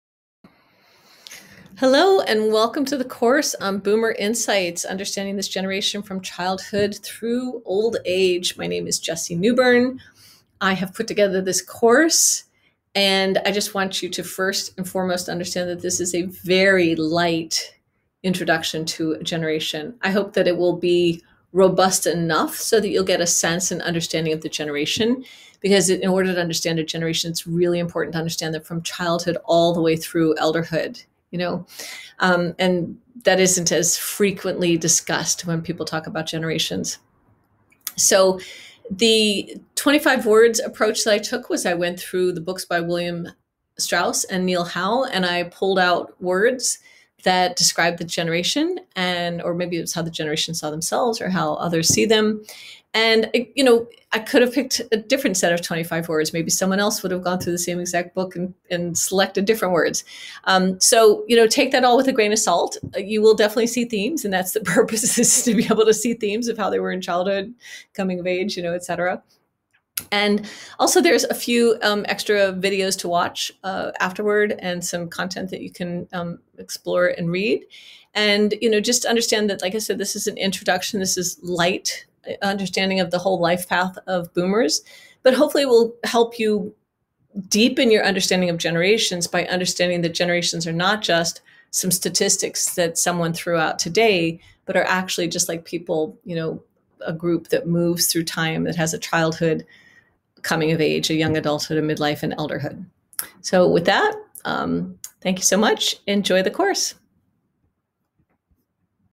Boomer Insights: Childhood to Old Age
In order to understand a generation, you need to understand it from childhood through elderhood. In this super short series, we'll look at the Boomer generation: their childhood, coming of age years, young adulthood, midlife and LEGENDARY elderhood.